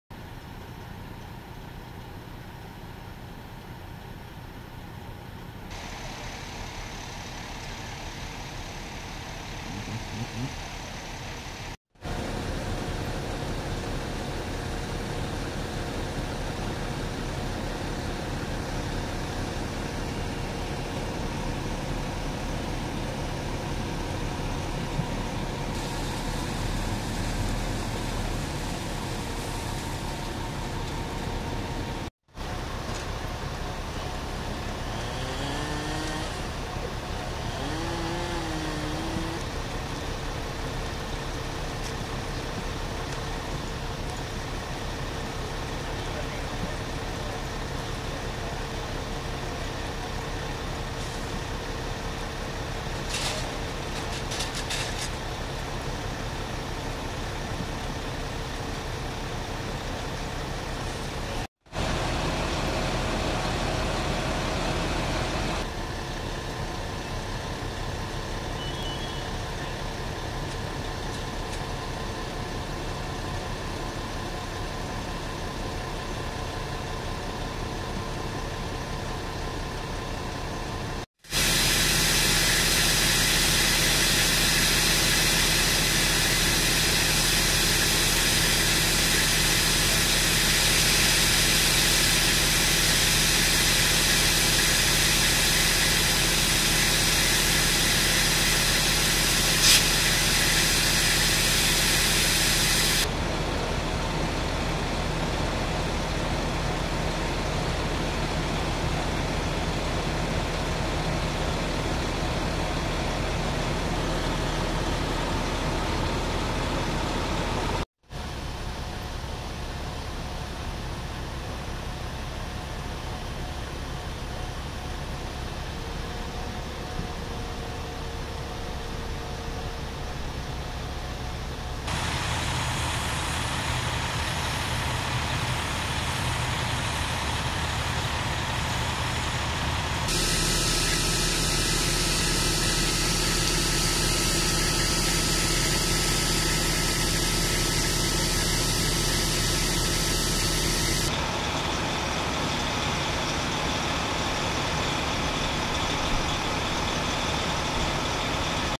Family and Pet Dog Displaced After House Fire - Poughkeepsie, New York
--- 3/31/18 -  POUGHKEEPSIE, DUTCHESS COUNTY ---

Early this morning a house fire broke out at 2170 New Hackensack Rd. Around 12:45AM DC911 had received a call of a fire in the bedroom of the home. The fire then quickly extended upwards. A second alarm was transmitted by Arlington Fire within moments.

Arlinton fire along with Mutual aid from New Hackensack had the blaze under control by approximately 1:20 AM

Assisting at the scene were the New Hamburg Fire Department, Mobile Life Support Services, Town of Poughkeepsie Police, Building Department, and Water Department, Dutchess County Department of Emergency Response and Central Hudson Gas and Electric.

No injuries were reported, A family of six along with their dog were displaced